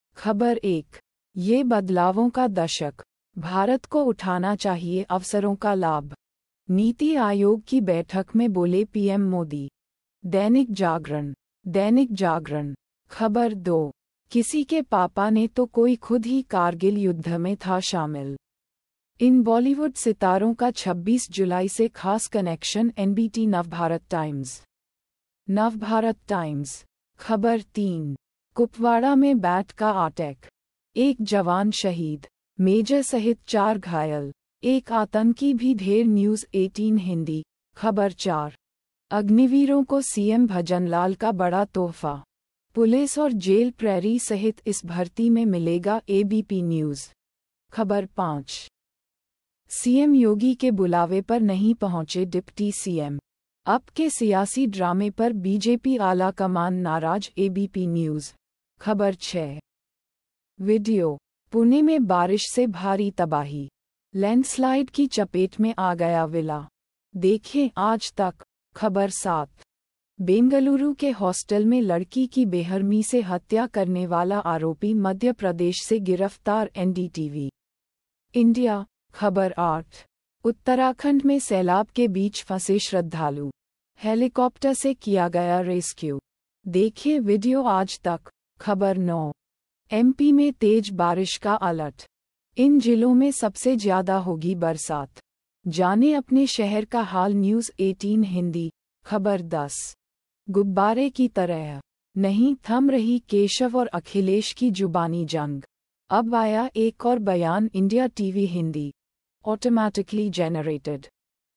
Hindi News (भारत)- 27/07/2024,06:01 PM GMT+5:30
ख़बर 1:'यह बदलावों का दशक, भारत को उठाना चाहिए अवसरों का लाभ', नीति आयोग की बैठक में बोले PM मोदी.. - दैनिक जागरण (Dainik Jagran)
ख़बर 2:किसी के पापा ने तो कोई खुद ही कारगिल युद्ध में था शामिल, इन बॉलीवुड सितारों का 26 जुलाई से खास कनेक्शन - NBT नवभारत टाइम्स (Navbharat Times)
ख़बर 3:कुपवाड़ा में BAT का आटैक, एक जवान शहीद, मेजर सहित चार घायल, 1 आतंकी भी ढेर - News18 हिंदी
ख़बर 4:अग्निवीरों को CM भजनलाल का बड़ा तोहफा, 'पुलिस और जेल प्रहरी सहित इस भर्ती में मिलेगा - ABP न्यूज़
ख़बर 5:CM योगी के बुलावे पर नहीं पहुंचे डिप्टी CM, UP के सियासी ड्रामे पर BJP आलाकमान नाराज - ABP न्यूज़
ख़बर 6:VIDEO: पुणे में बारिश से भारी तबाही, लैंडस्लाइड की चपेट में आ गया विला, देखें - Aaj Tak
ख़बर 7:बेंगलुरु के हॉस्टल में लड़की की बेहरमी से हत्या करने वाला आरोपी मध्य प्रदेश से गिरफ्तार - NDTV India
ख़बर 8:उत्तराखंड में सैलाब के बीच फंसे श्रद्धालु, हेलिकॉप्टर से किया गया रेस्क्यू, देखें VIDEO - Aaj Tak
ख़बर 9:MP में तेज बारिश का अलर्ट, इन जिलों में सबसे ज्यादा होगी बरसात; जानें अपने शहर का हाल - News18 हिंदी
ख़बर 10:'गुब्बारे की तरह...', नहीं थम रही केशव और अखिलेश की जुबानी जंग, अब आया एक और बयान - India TV Hindi

Automatically Generated...